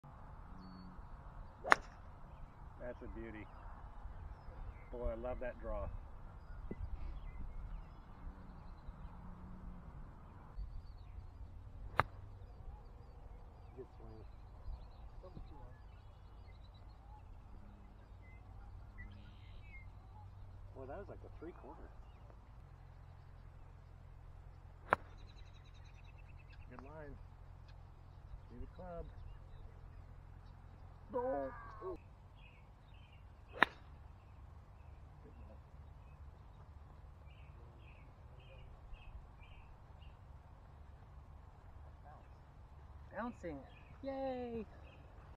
Beautiful day of golf at Quail Valley Golf Course.
Quail Valley is a beautiful course on the edge of the Portland metro area just off the east end of Highway 6. Mostly wide open fairways, smaller greens that are in excellent shape and views of farmland all around. Priced moderately but a deal compared to nearby Pumpkin Ridge. Flat terrain that is walkable.